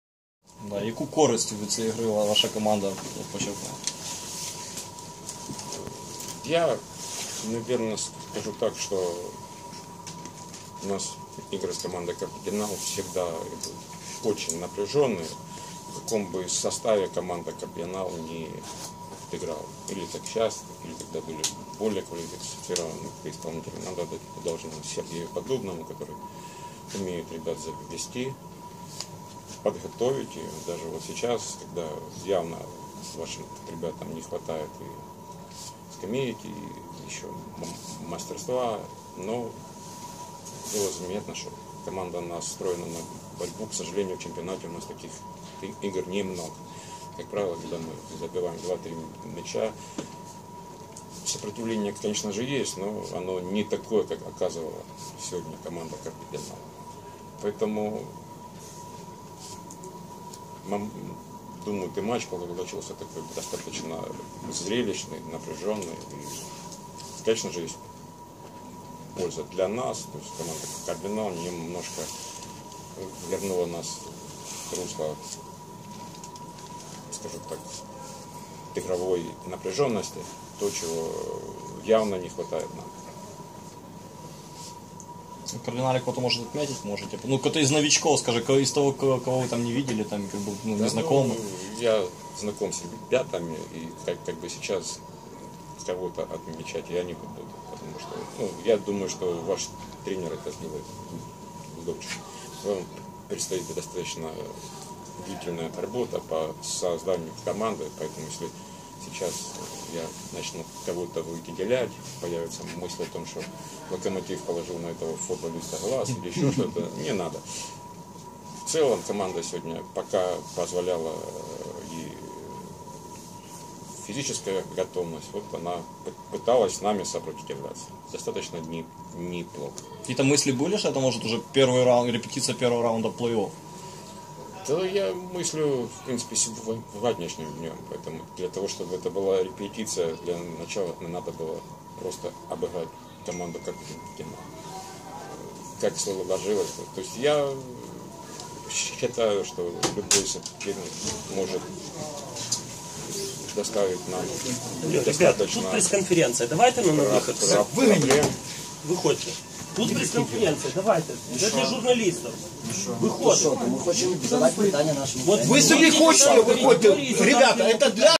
Ривкін: «Кардинал» повернув нас до русла ігрової напруженості»
Післяматчеві коментарі головного тренера харківського міні-футбольного клубу "Локомотив" Євгена Ривкіна. Матч Кардинал-Локомотив завершився перемогою гостей - 2-5.